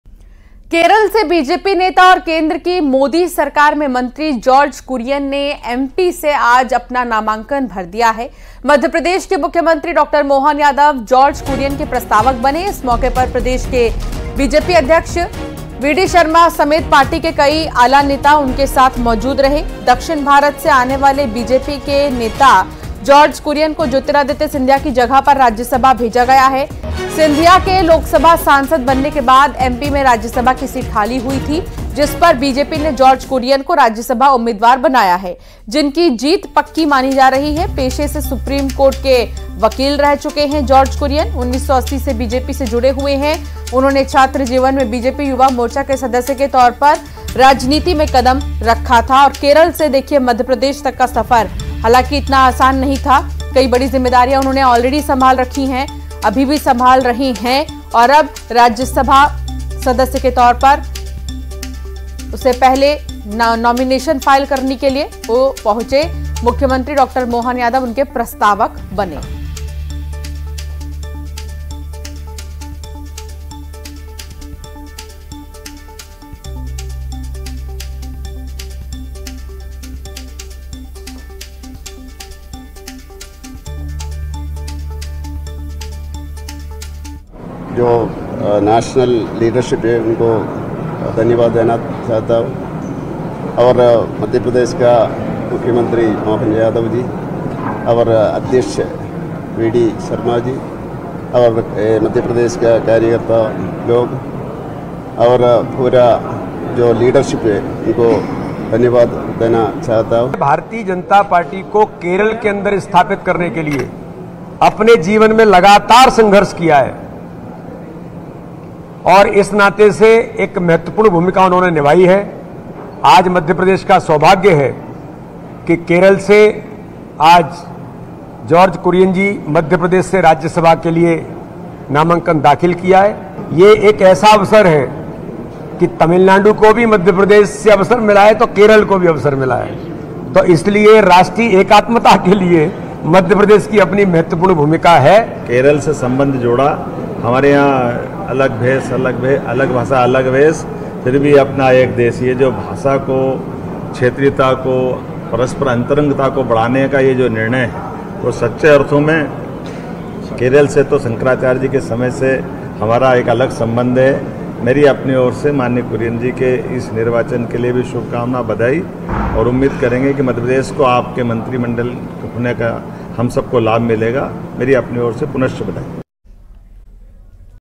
George Kurian Nomination News: जॉर्ज कुरियन ने भरा राज्यसभा का नामांकन | CM Mohan Yadav | MP News
बीजेपी के राज्यसभा उम्मीदवार जार्ज कुरियान ने विधानसभा में नामांकन दाखिल कर दिया. इस दौरान उनके साथ मुख्यमंत्री डॉ. मोहन यादव और बीजेपी के प्रदेश अध्यक्ष वीडी शर्मा भी थे. गौरतलब है कि यह सीट केंद्रीय मंत्री ज्योतिरादित्य की वजह से खाली हुई थी. उन्होंने लोकसभा चुनाव में गुना सीट से जीत दर्ज की थी. इसलिए इस सीट पर चुनाव होना है. कुरियन का कार्यकाल 9 अप्रैल 2026 तक होगा. राज्यसभा चुनाव विधानसभा में 3 सितंबर को होगा. विधानसभा के समीकरण बताते हैं कि कुरियन का मध्य प्रदेश से राज्यसभा लगभग तय है.